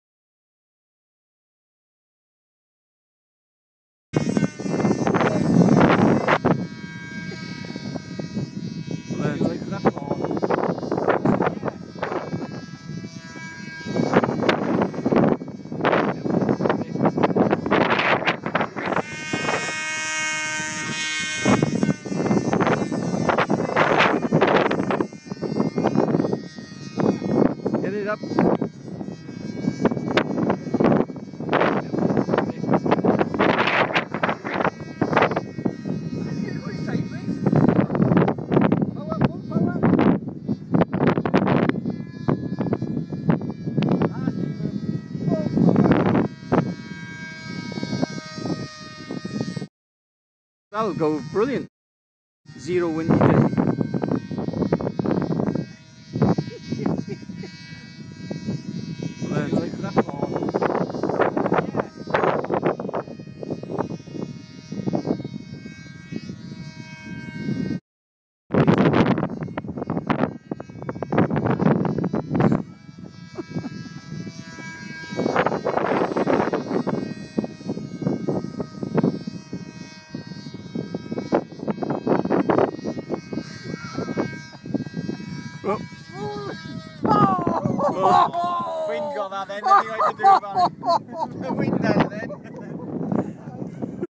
RC Sidewinder AIM 9 Missile Home Made from Depron Test Flight & Crash
Quick test flight of home made depron Missile
Not good test flight conditions but i couldn't wait till a nice day to test it
winds are 12-16mph
Visibility good
Steve camera man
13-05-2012

Nose damaged on impacted but all fixed in 10 minutes when i got home